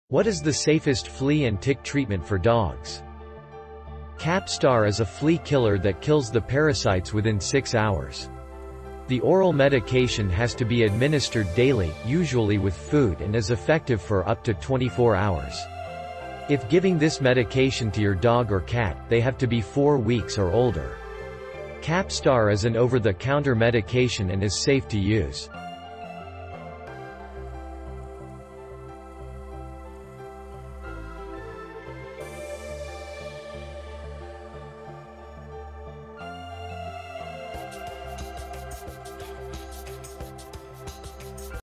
What is the safest flea and tick treatment for dogs?
Capstar is a flea killer that kills the parasites within 6 hours. The oral medication has to be administered daily, usually with food and is effective for up to 24 hours. If giving this medication to your dog or cat, they have to be 4 weeks or older. Capstar is an over the counter medication and is safe to use.

Reference:
Best Flea and Tick Treatment for Dogs: The Ultimate 2019 Guide ...